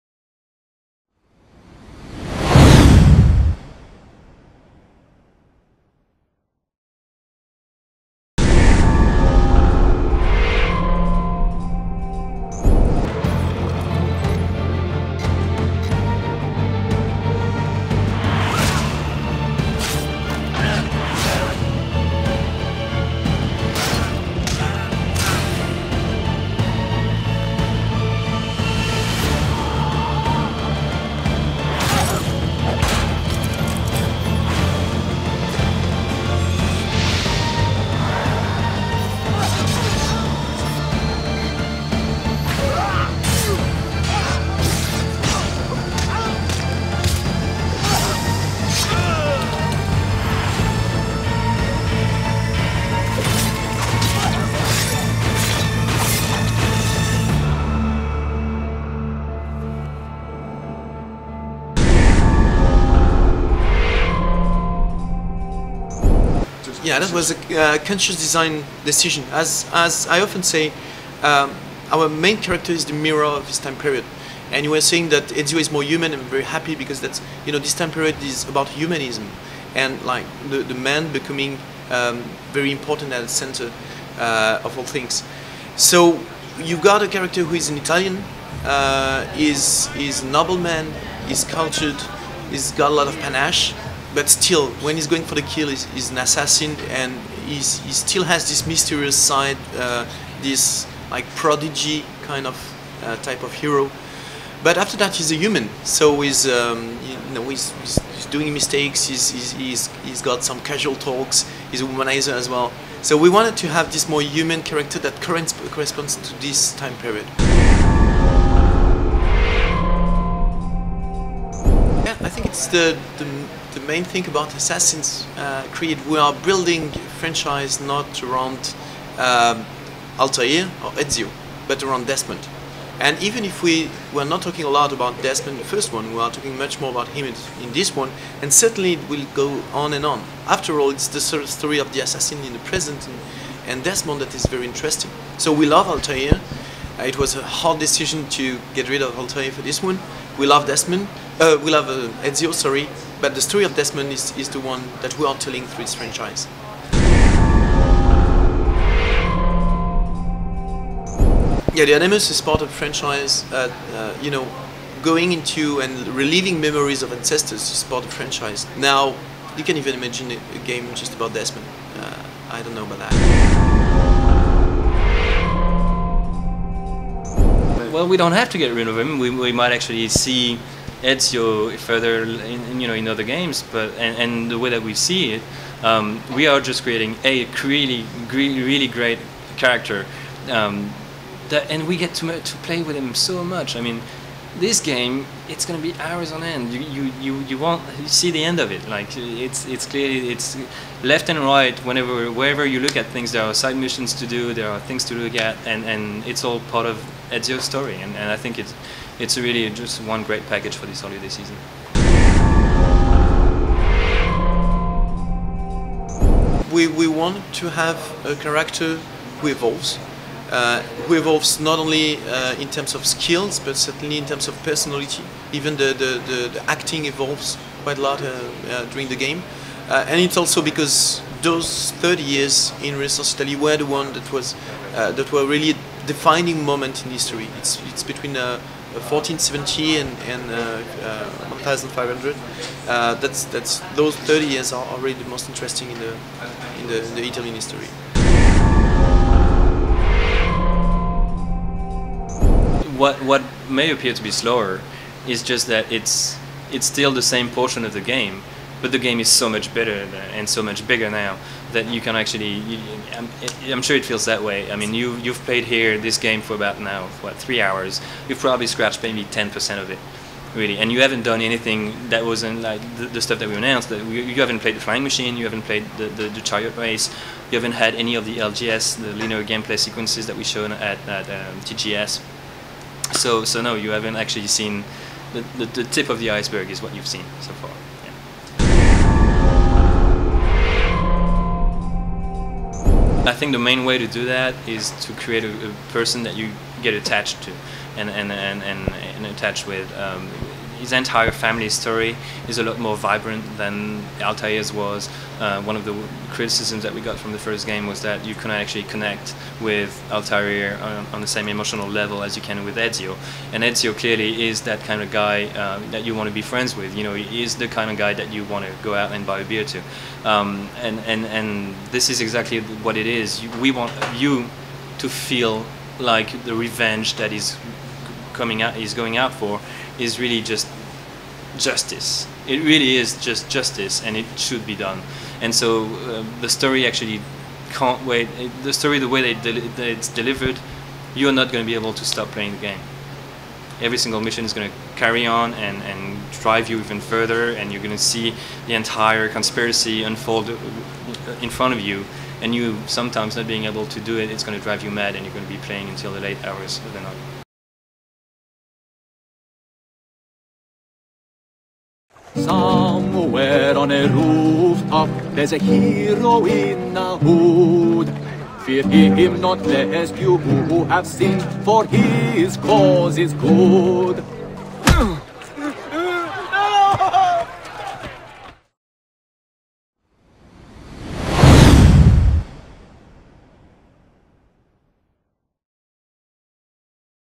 Enter The Creed: The AC2 Interviews: Episode 2: The Characters
Episode 2 of a 5 part video feature where we meet various members of the Assassin's Creed 2 development team and talk EVERYTHING Assassin's Creed.